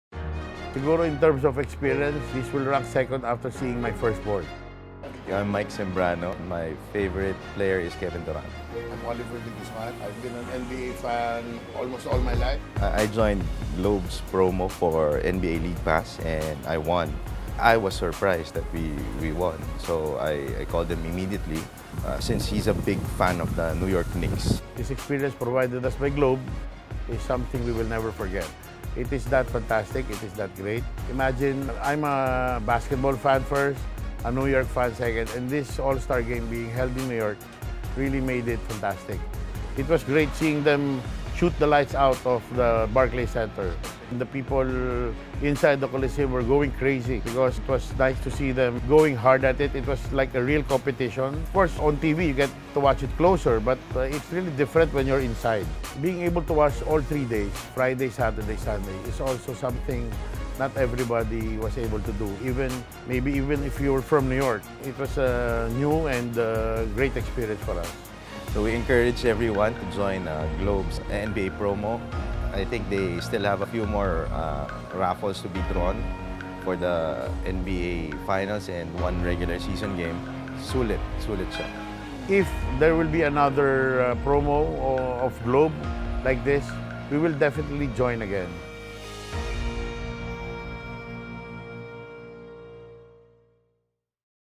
GlobePHNBA Live in New York
It doesn't get much better than watching the NBA COURTSIDE! LetsTakeOverSummer with GlobePHNBA

Don’t pass up the chance to watch the Finals live in the US.